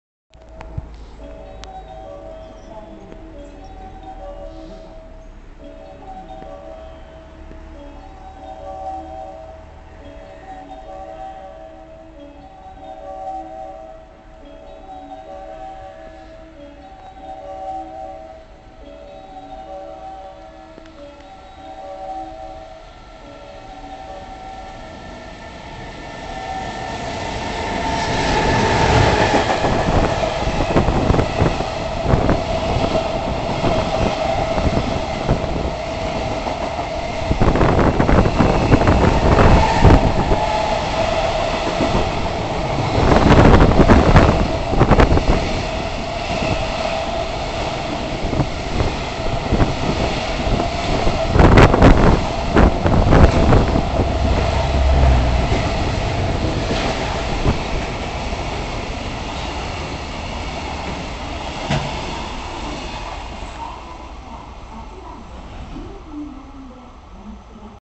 EF210-164牽引貨物列車 EF210-106牽引貨物列車同時通過‼ 魚住駅にて
サブチャンネルもよろしく！